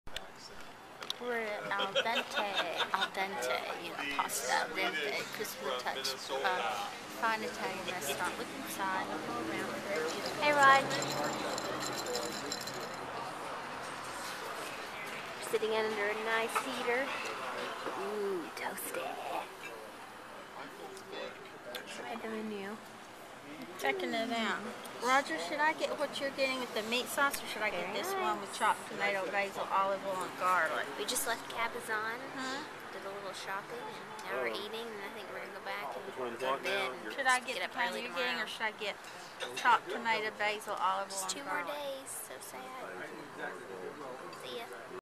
Palm Springs Day 4 Pt. 4
Dinner at a new place for us... Al Dente... Great little pasta place. I had the special of the night, lasagna... Mom got risotto with mushrooms and Dad got the spaghetti with marinara and basil. Such a wonderful little atmosphere!